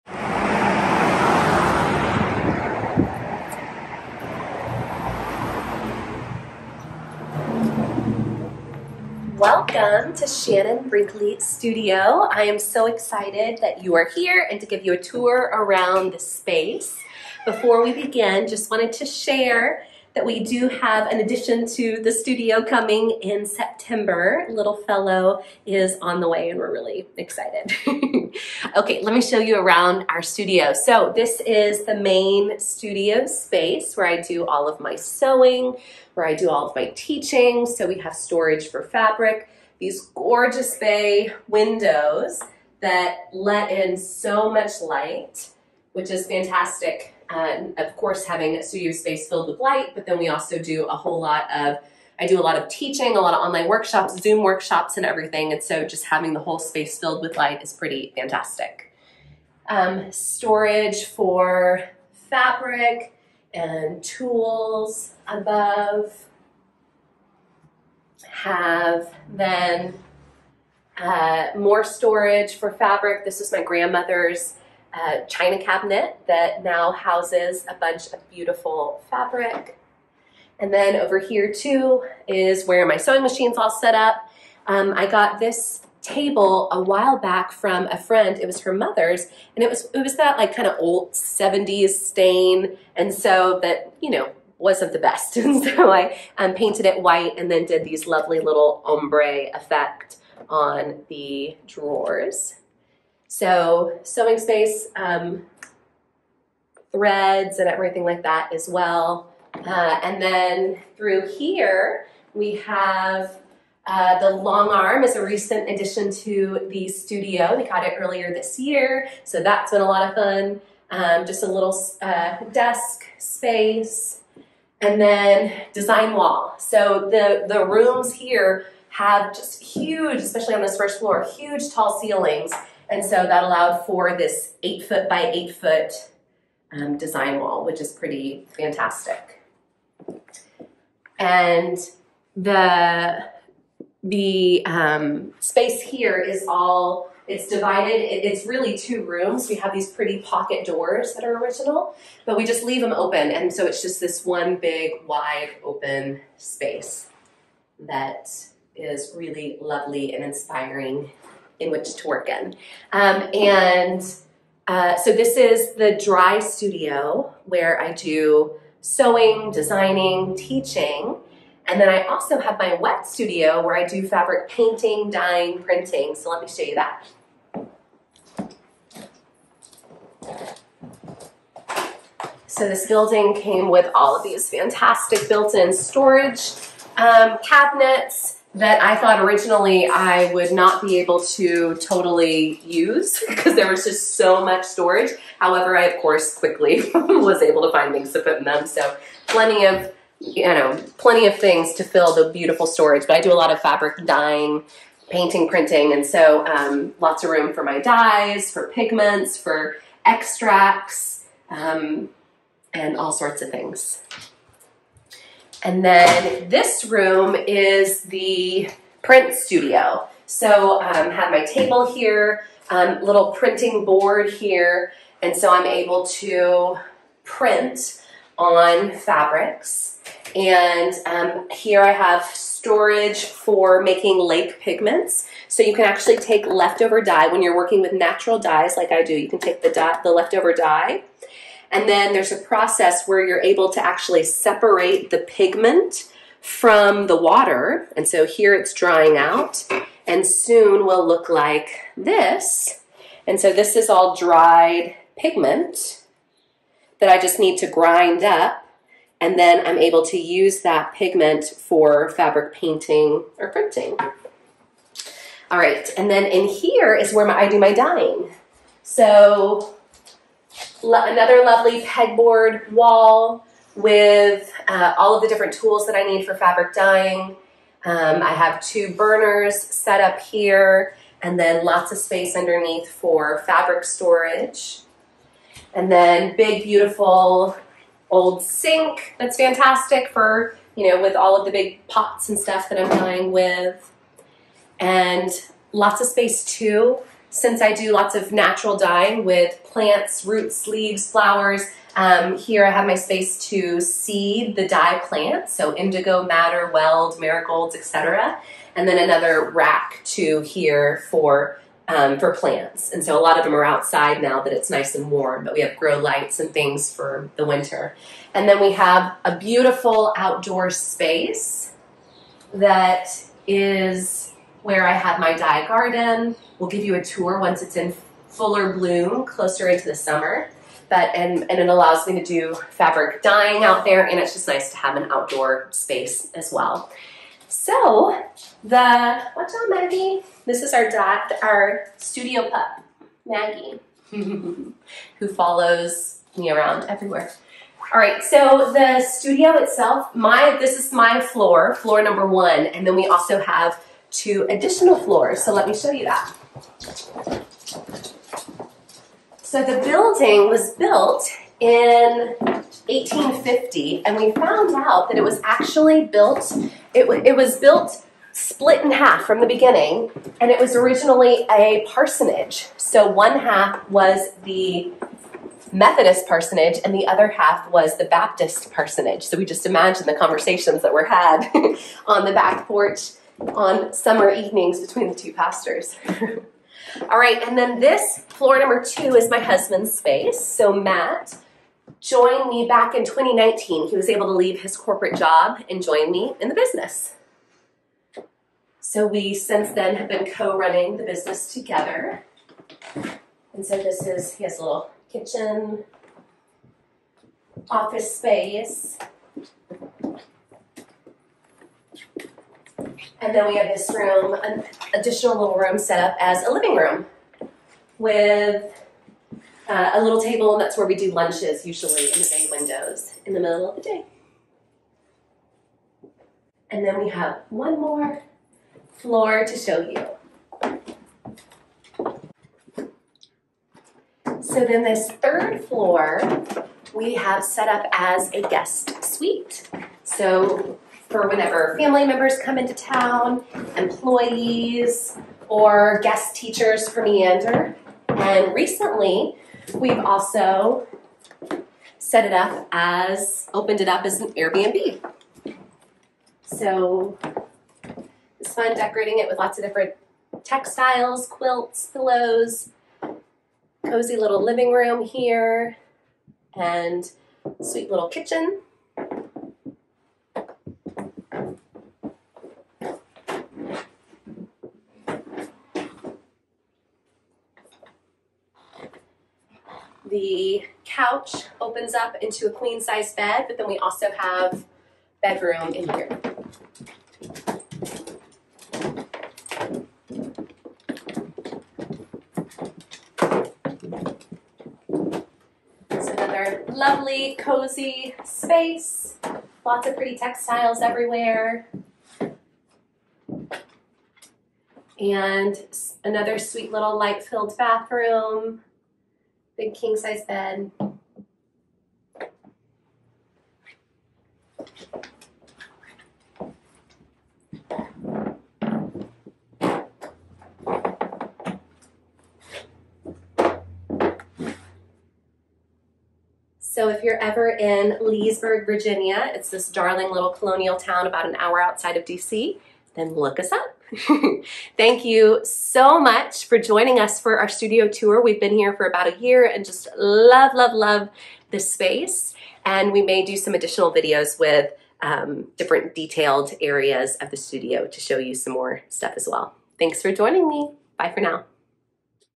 Shannon Brinkley Quilt Studio Tour!
Come take a tour around our Quilt Studio!

We've been in this space in downtown Leesburg for just over a year and are loving it! It is a short 5 min walk from our home and is a stones throw from lots of fantastic restaurants, pubs, and shops!

The building was built in 1850 and has super tall ceilings, bay windows, crown molding, and so many original features.